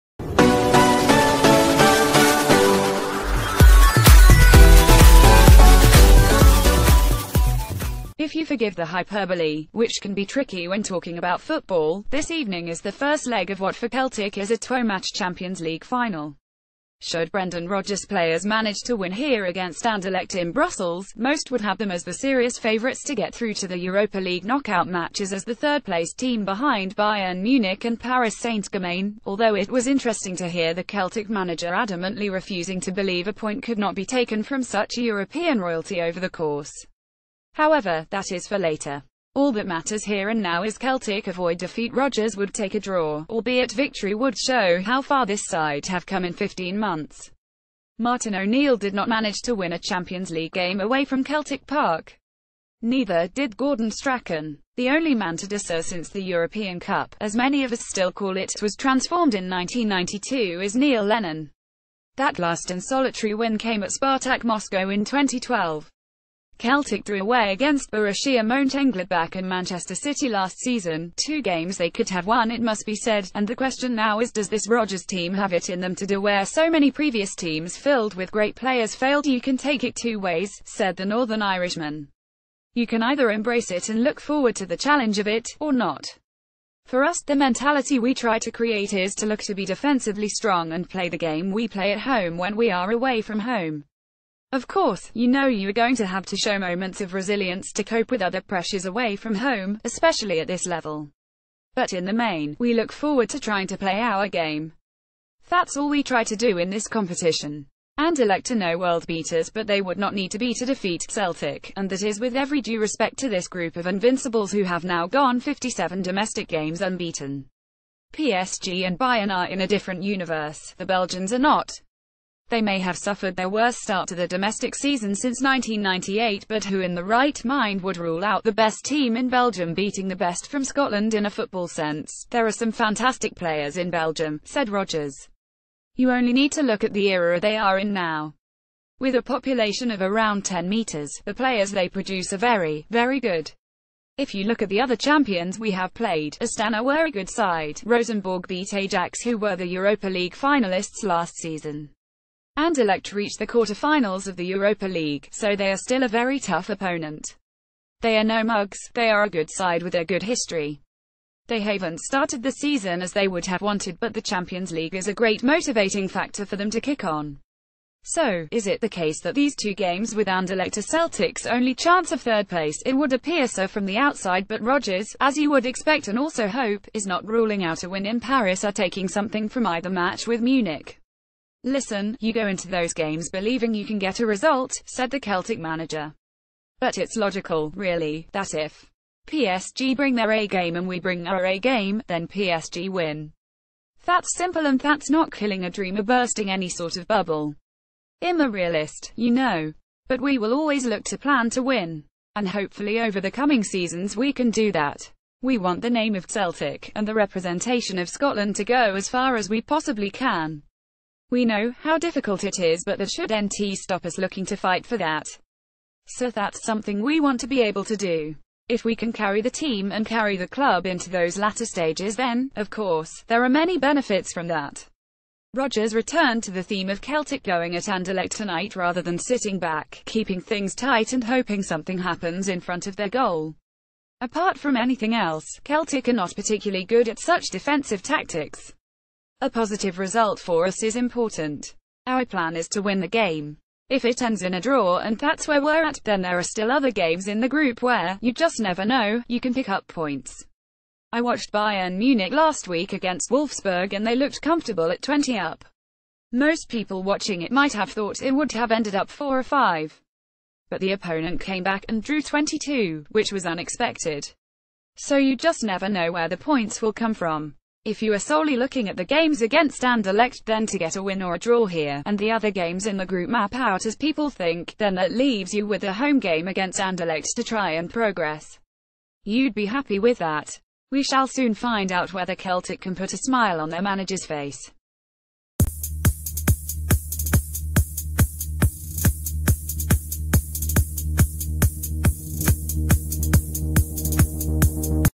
[NEWS 24h] Rodgers vows to go where few celtic managers have gone before
Rodgers vows to go where few celtic managers have gone before
IF you forgive the hyperbole, which can be tricky when talking about football, this evening is the first leg of what for Celtic is a twomatch Champions League final. Should Brendan Rodgers’s players m...
facebook friends 4 ways!